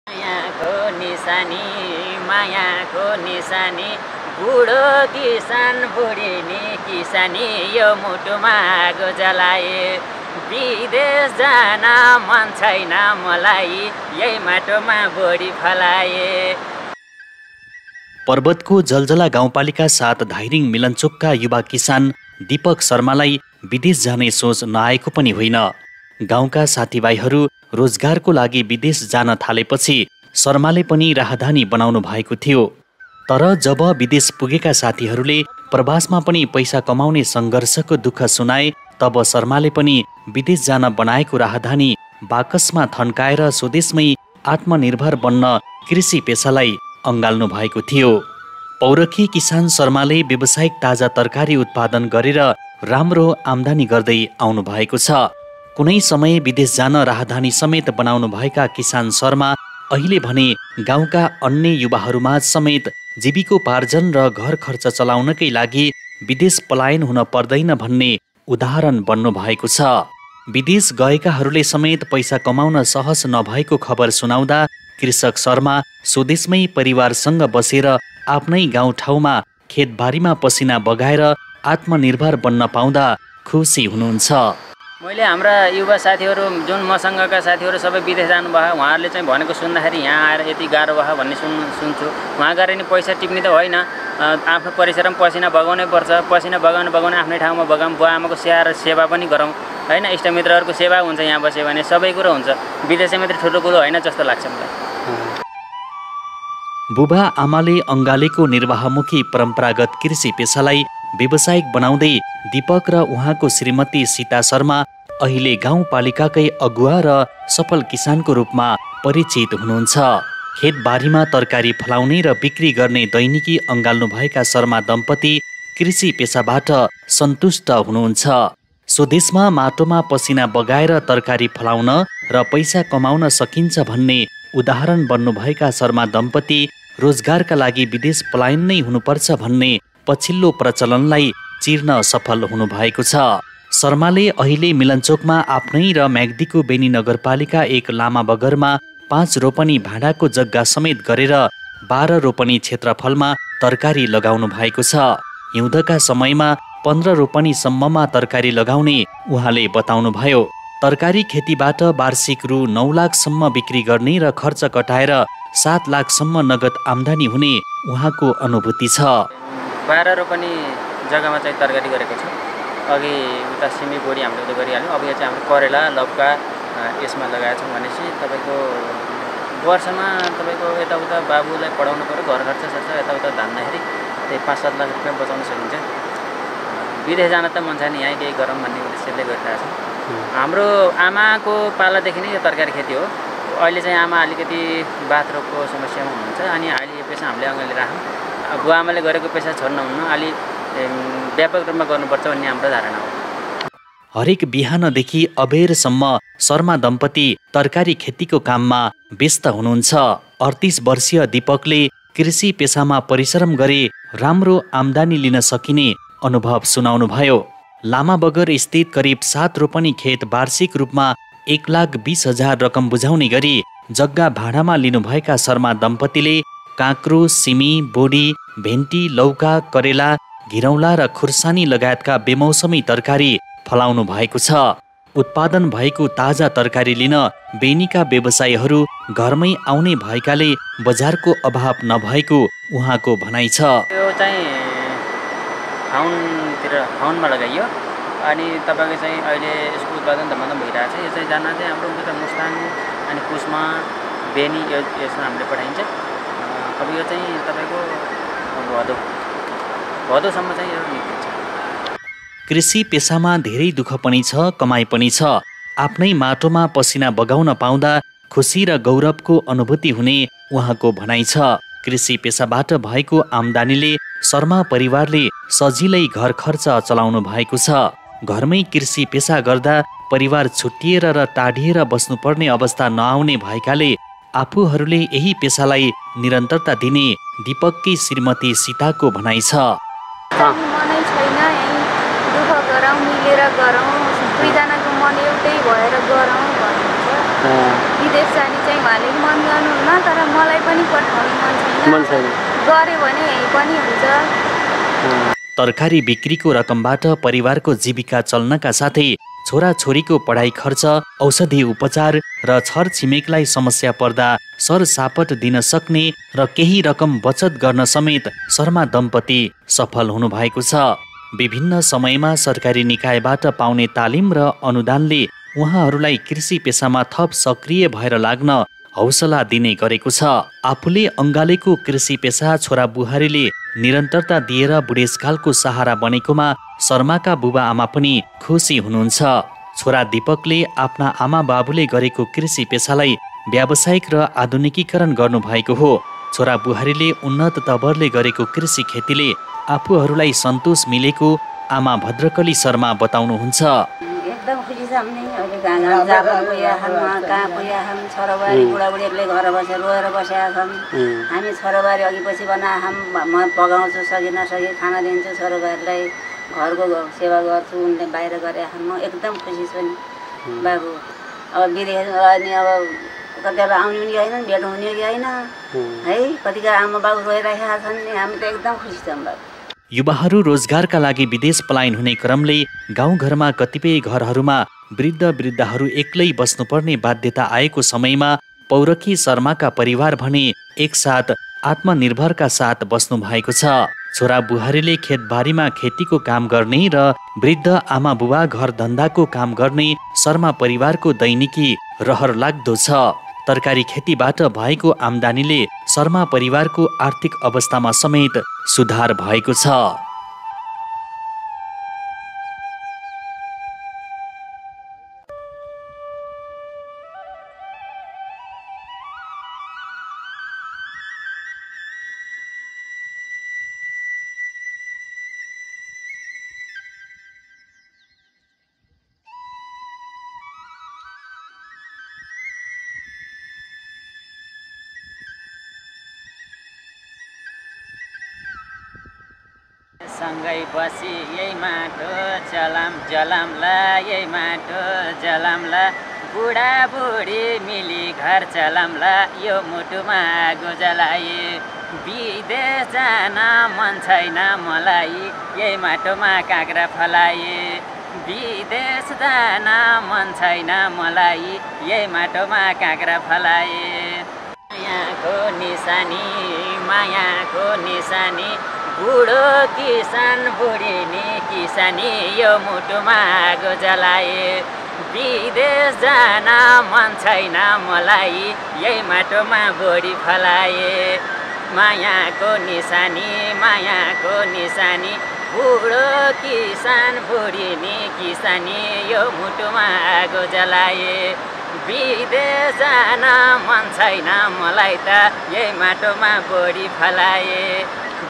‘पौरखी दम्पती, खुसी परिवार’ || तरकारी खेतीबाट रहरलाग्दो कमाइ || The Success Story
जलजला गाउँपालिका ७ धाइरिङ मिलनचोकका दिपक आचार्यले कृषि खेतीमा प्राप्त गरेको सफलताको कथा समेटिएको भिडियो स्टोरी ।